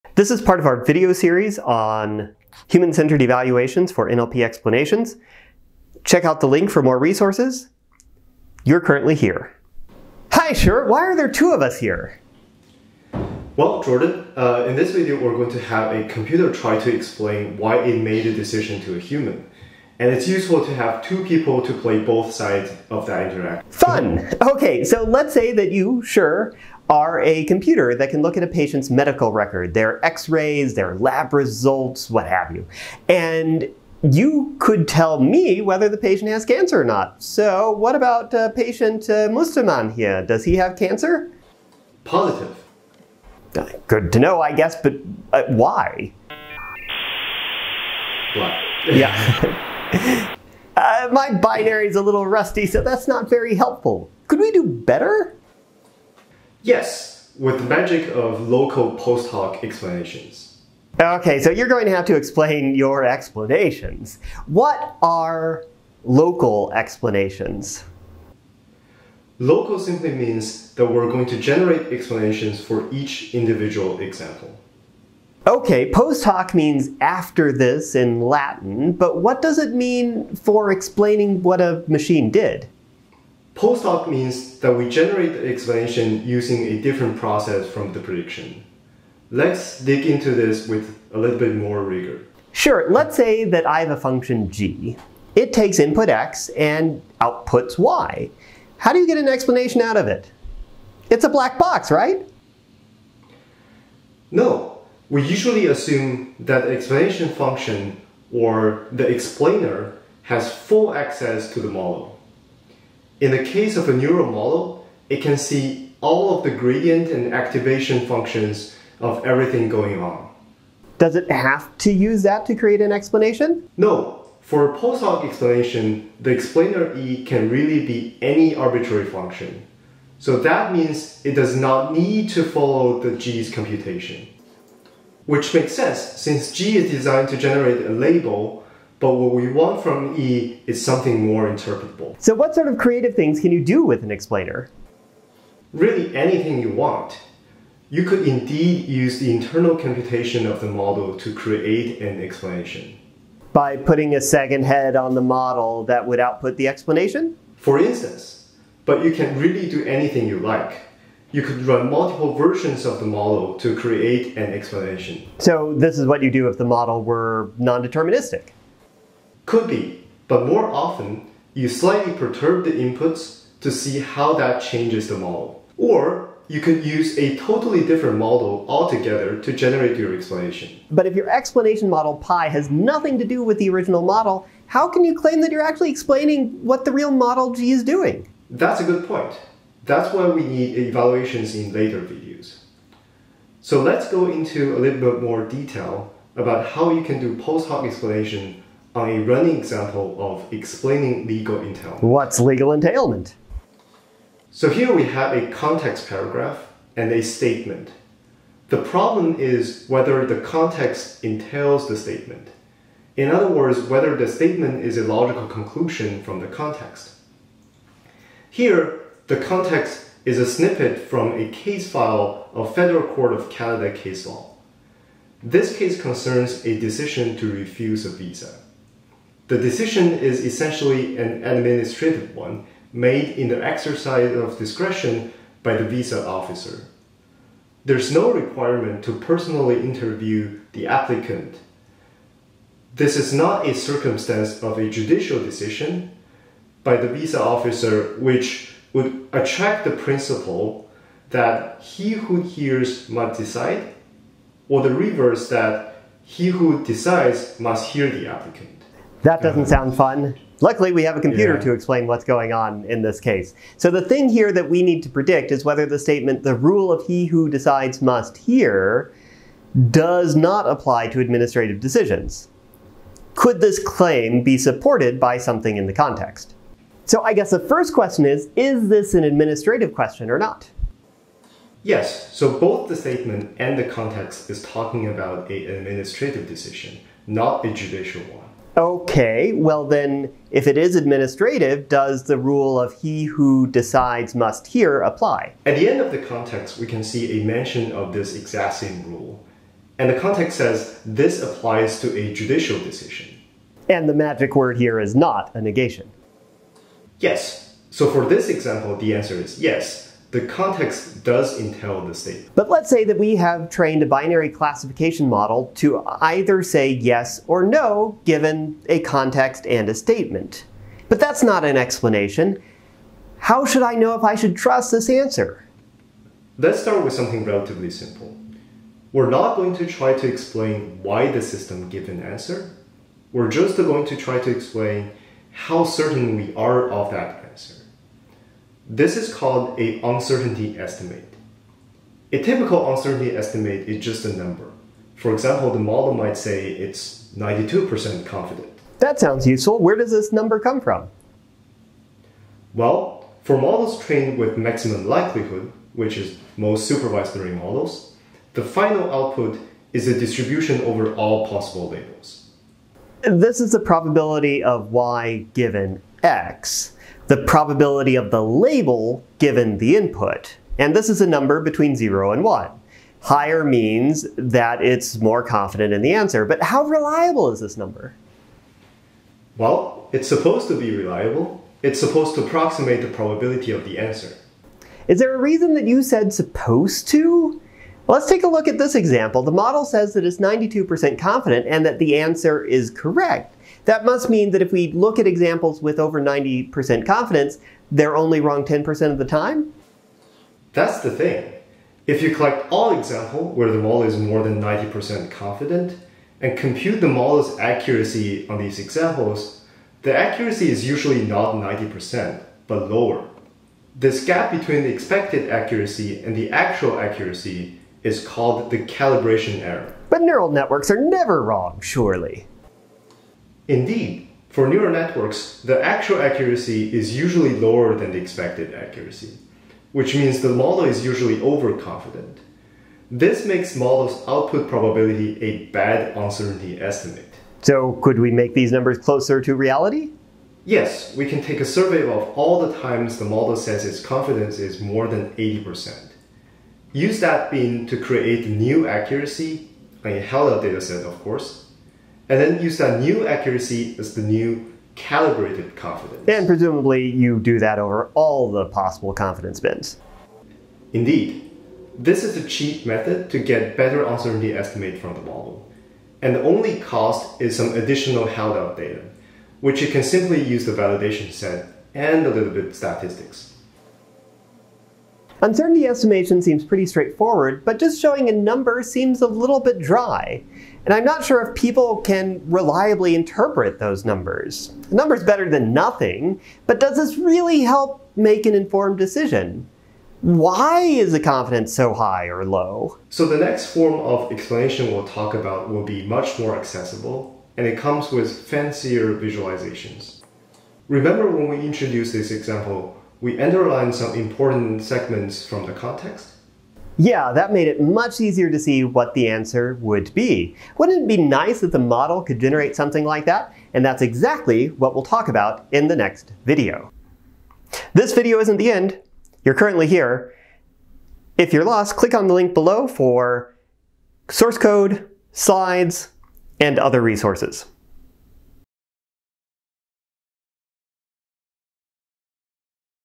Explaining AI Decisions: Calibration [Lecture]
This is part of a video series from our NAACL tutorial on Human-Centered Evaluations of Explanations.